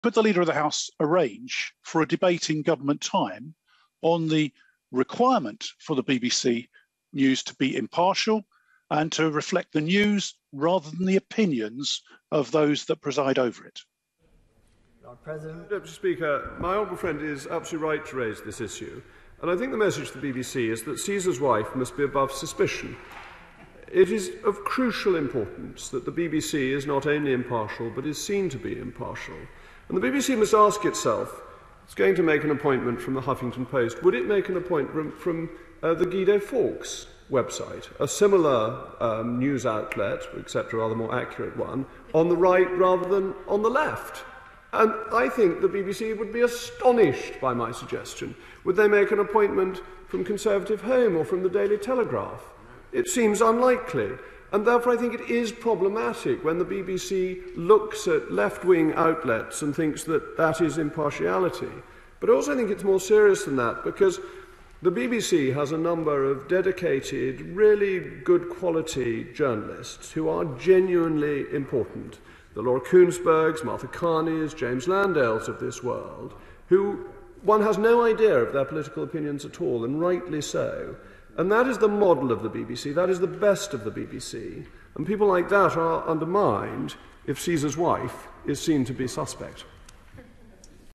Rees-Mogg erupts at "left-wing BBC" in fiery Commons rant over 'problematic' hiring plans
Jacob Rees-Mogg has said it would be "problematic" for the BBC to appoint a journalist from a "left-wing" outlet to a role as a news editor. Ex-Huffington Post UK editor Jess Brammar is reportedly under consideration for a role to oversee the corporation's news channels. The Cabinet minister has recently suggested the BBC is damaging its reputation for impartiality by consistently making senior appointments from people on the political left and not the right.